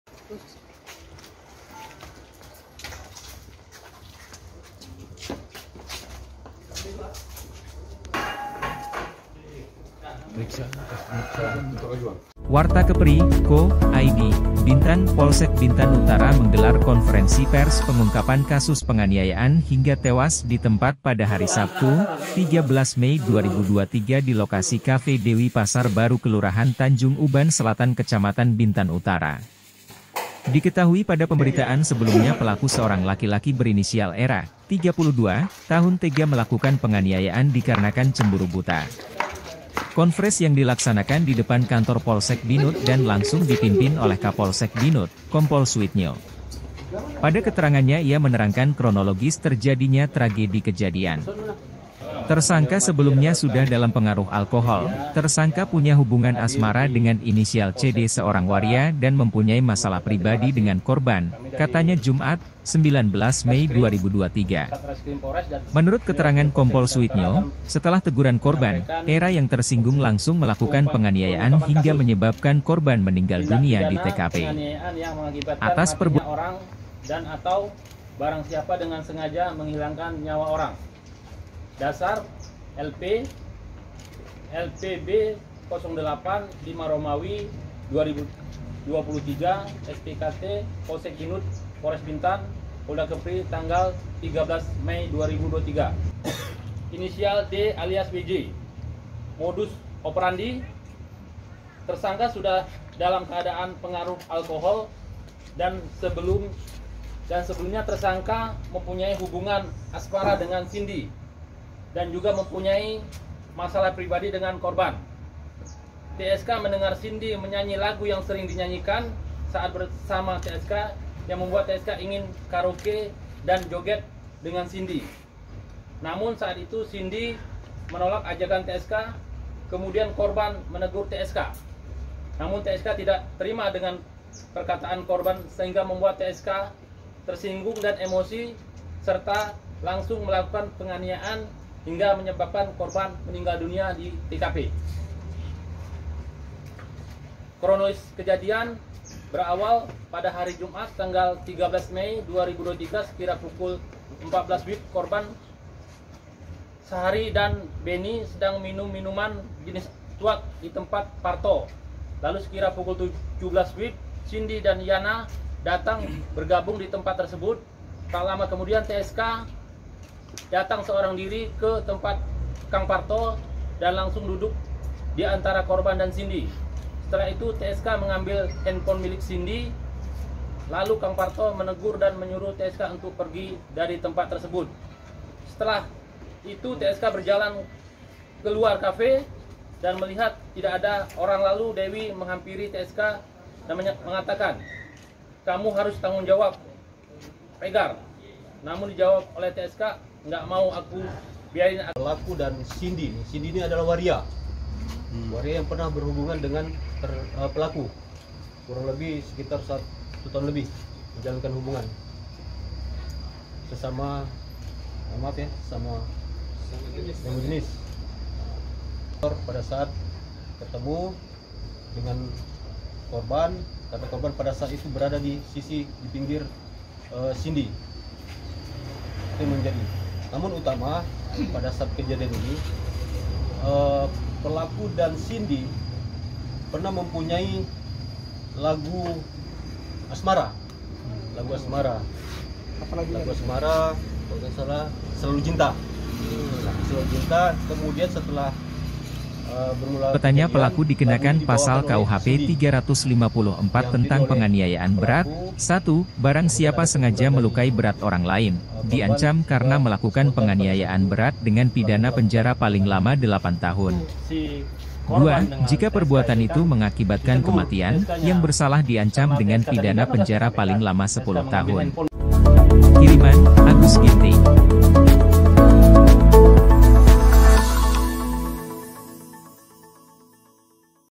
Era Cemburu Buta Dengan Seorang Waria, Kronologi Kasus Penganiayaan di Cafe Dewi Bintan
wartakepritv Era Cemburu Buta Dengan Seorang Waria, Kronologi Kasus Penganiayaan di Cafe Dewi